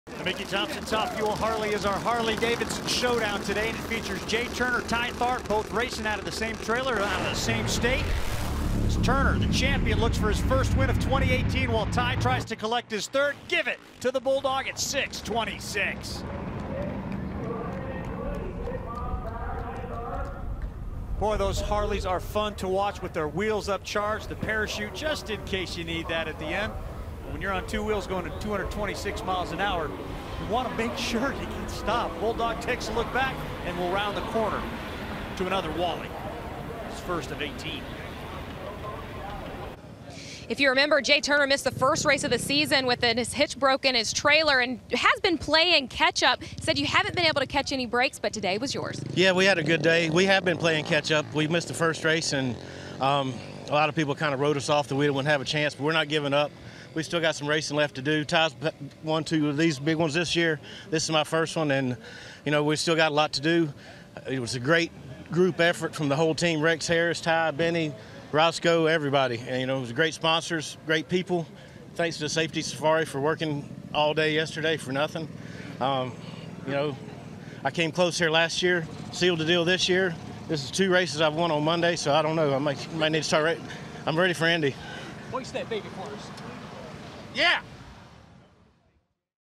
HeartlandNats: Top Fuel Harley winner Jay Turner
The Bulldog Jay Turner took home his first win of 2018 at the Menards HeartlandNats presented by Minties!

Mickey Thompson Tires Top Fuel Harley